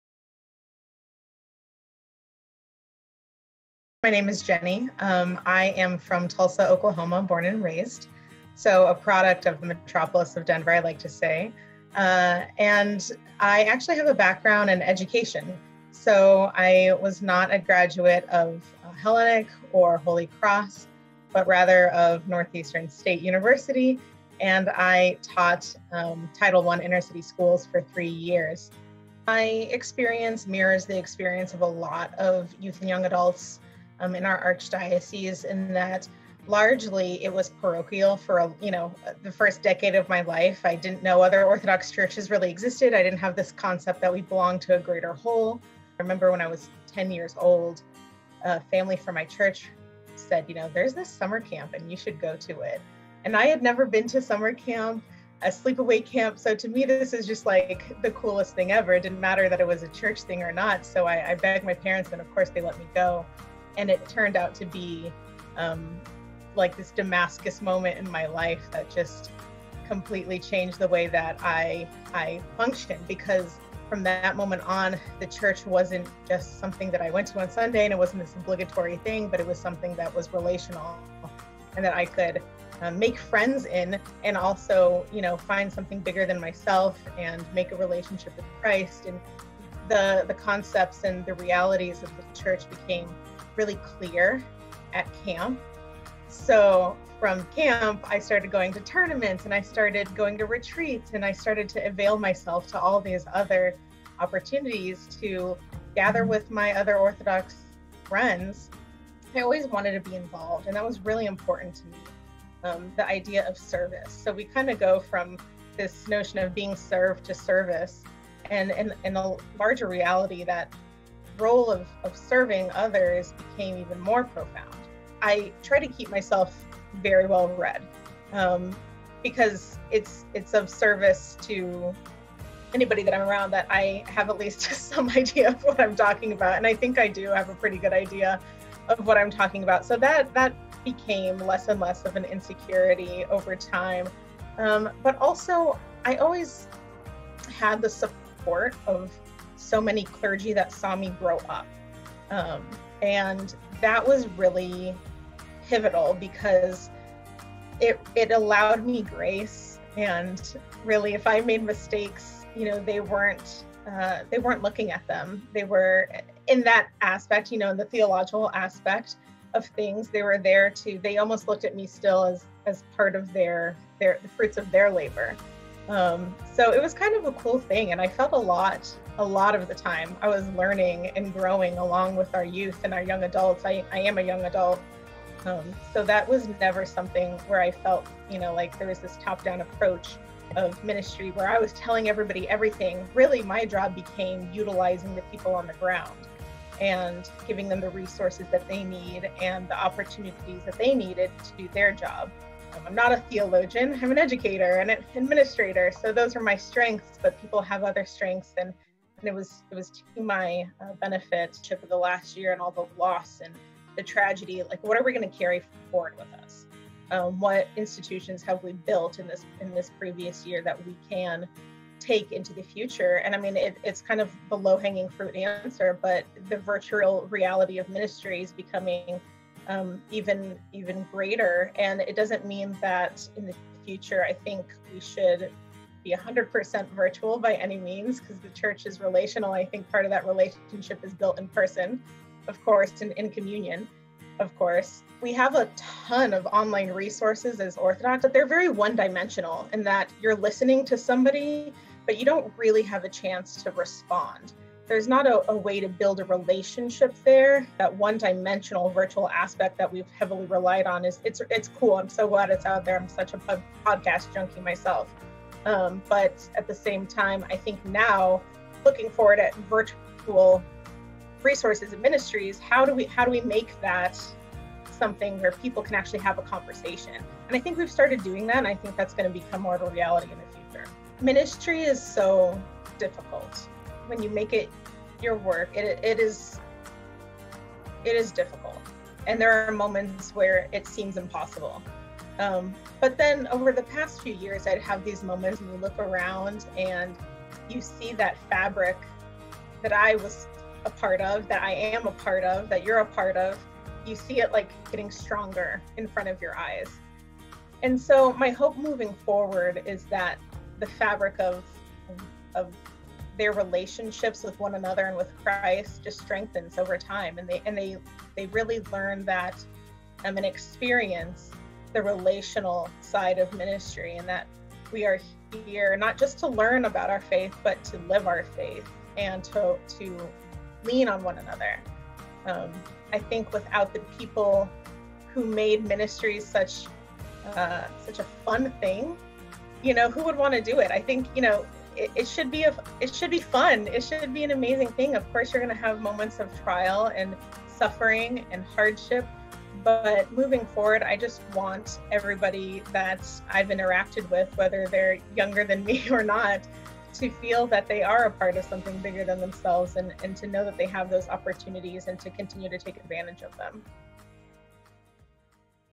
Orthodox Observer Interview: Jenny Stasinopoulos
After 5 years of service as the Director of Youth and Young Adult Ministries for the Metropolis of Denver, Jenny Stasinopoulos is moving on to her next role with the Tulsa Children's Museum. The Orthodox Observer caught up with Jenny to reflect on her time in youth ministry, the future of youth work, and her hopes for all the amazing youth and young adults she served.